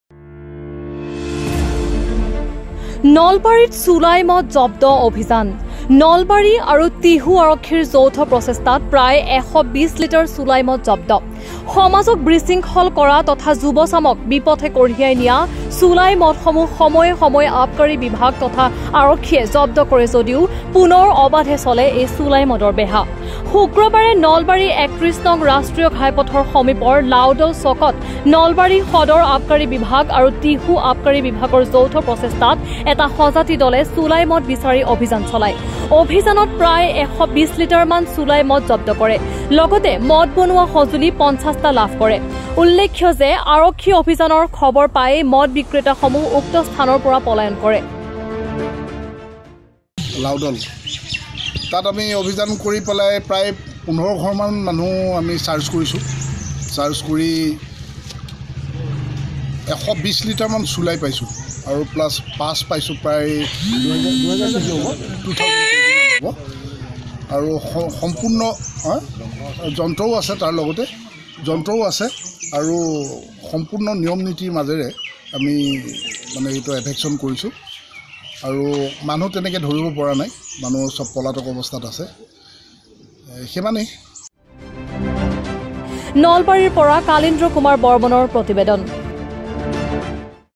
নলবাৰীত চুলাই মদ জব্ধ অভিযান।
নলবাৰী আৰু টিহু আৰক্ষীৰ যৌথ প্ৰচেষ্টাত প্ৰায় ১২০ লিটাৰ চুলাই মদ জব্ধ।